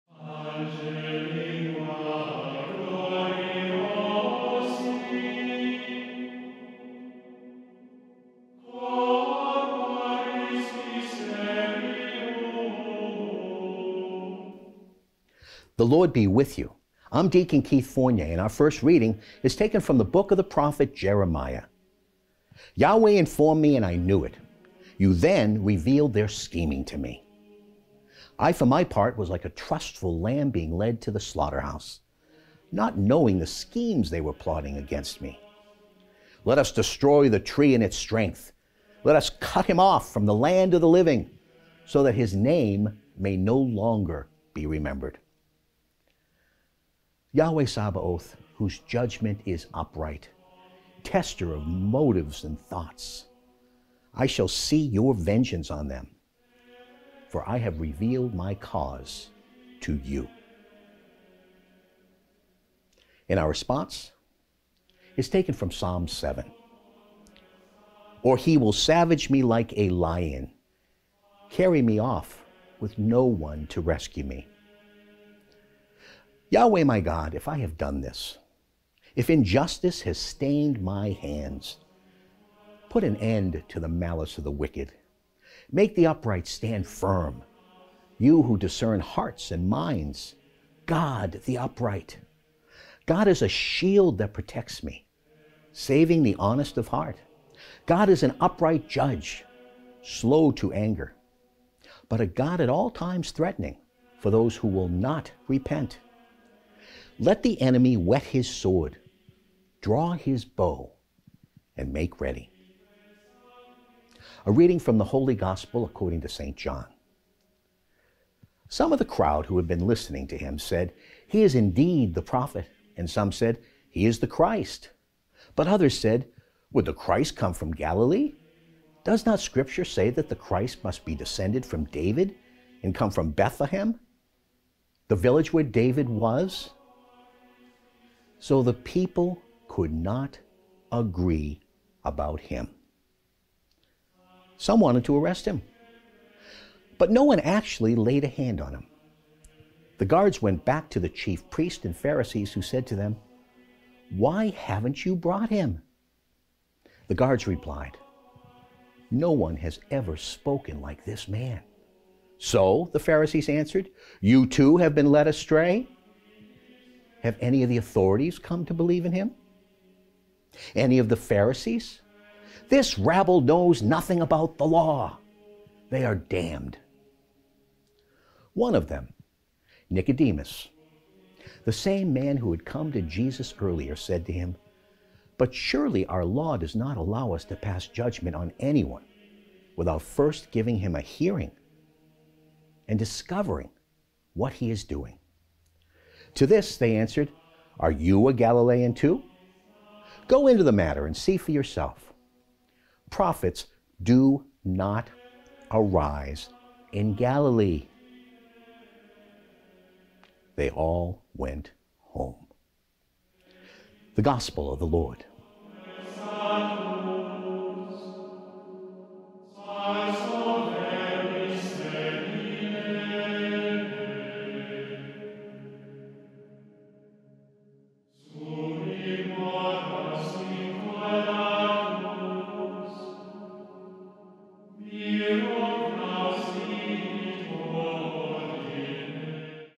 Daily Reading for Saturday, April 2nd, 2022 HD
Reading 1, Jeremiah 11:18-20
18 Yahweh informed me and I knew it; you then revealed their scheming to me.

19 I for my part was like a trustful lamb being led to the slaughterhouse, not knowing the schemes they were plotting against me, 'Let us destroy the tree in its strength, let us cut him off from the land of the living, so that his name may no longer be remebered!'

20 Yahweh Sabaoth, whose judgement is upright, tester of motives and thoughts, I shall see your vengeance on them, for I have revealed my cause to you.

2 or he will savage me like a lion, carry me off with no one to rescue me.

3 Yahweh my God, if I have done this: if injustice has stained my hands,

9 Put an end to the malice of the wicked, make the upright stand firm, you who discern hearts and minds, God the upright.

10 God is a shield that protects me, saving the honest of heart.

11 God is an upright judge, slow to anger, but a God at all times threatening

12 for those who will not repent. Let the enemy whet his sword, draw his bow and make ready;

40 Some of the crowd who had been listening said, 'He is indeed the prophet,'

41 and some said, 'He is the Christ,' but others said, 'Would the Christ come from Galilee?

42 Does not scripture say that the Christ must be descended from David and come from Bethlehem, the village where David was?'

43 So the people could not agree about him.

44 Some wanted to arrest him, but no one actually laid a hand on him.

45 The guards went back to the chief priests and Pharisees who said to them, 'Why haven't you brought him?'

46 The guards replied, 'No one has ever spoken like this man.'

47 'So,' the Pharisees answered, 'you, too, have been led astray?

48 Have any of the authorities come to believe in him? Any of the Pharisees?

49 This rabble knows nothing about the Law -- they are damned.'

50 One of them, Nicodemus -- the same man who had come to Jesus earlier -- said to them,

51 'But surely our Law does not allow us to pass judgement on anyone without first giving him a hearing and discovering what he is doing?'

52 To this they answered, 'Are you a Galilean too? Go into the matter, and see for yourself: prophets do not arise in Galilee.'

53 They all went home,